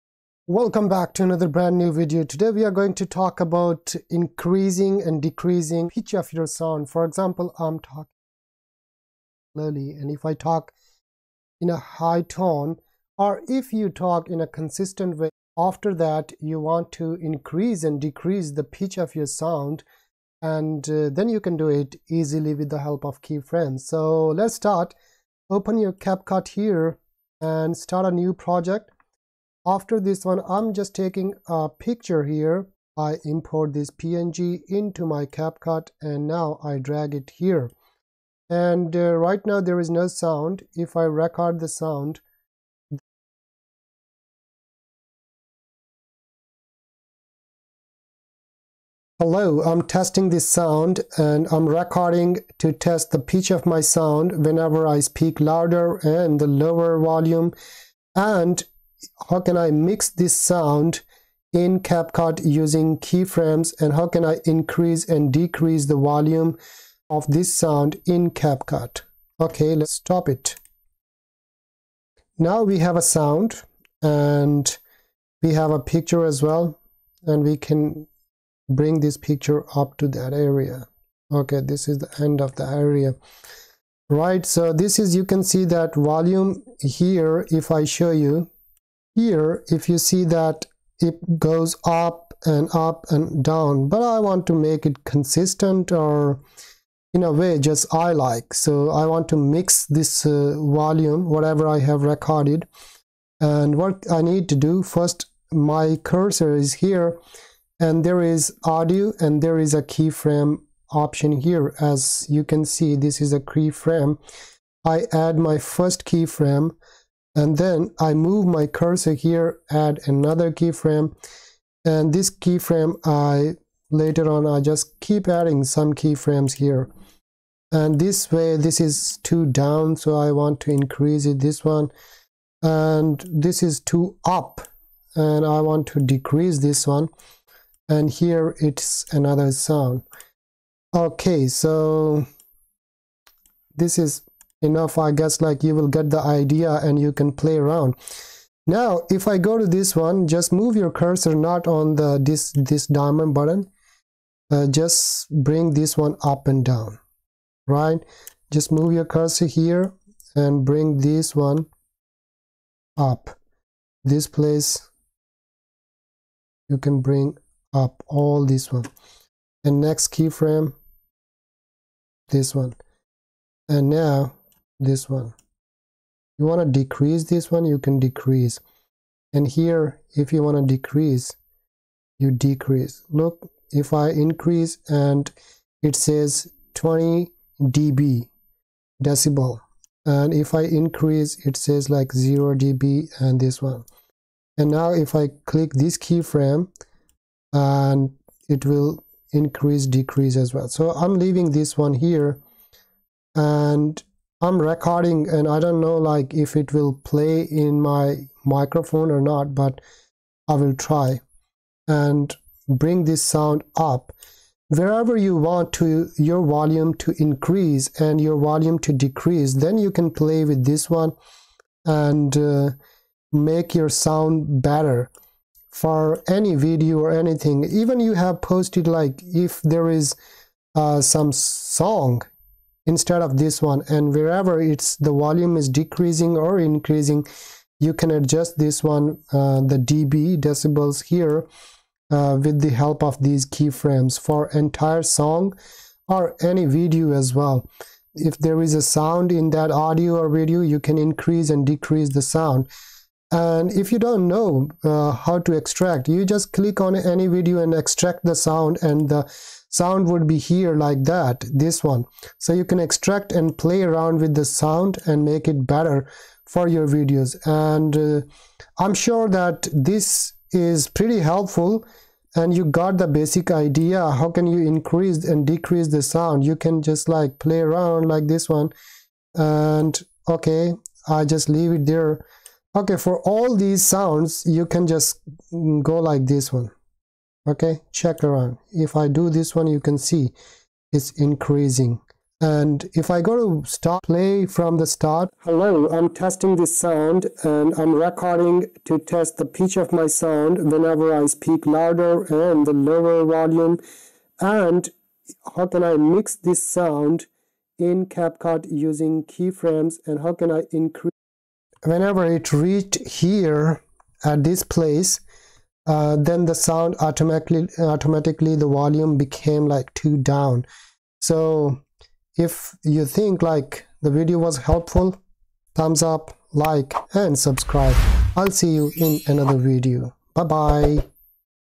Learn How to Adjust Audio Levels With Keyframes in CapCut (Audio Ducking)
In this video tutorial, you will learn how to edit audio in CapCut PC with Keyframes. You can use audio autoduck function for low and high volume adjustments in CapCut. Editing audio with keyframes in Capcut is a great way to adjust the volume levels in your video. If you are using voiceover and background music for your CapCut Videos then you definitely need audio ducking to balance the unequal levels of audio. This task can be performed within CapCut with the help of audio editing keyframes.
If you use an already existing sound or your own audio voiceover with the background music or a song, then you must know how to adjust the pitch or the unequal levels of audio to match voiceover and background music with the help of CapCut PC keyframes. It is necessary to keep the balance of different voices and adjust volume of background music bit higher whenever you take a pause.

You can follow the same procedure for CapCut mobile to make the audio levels equal or low and high with the keyframes for volume adjustments.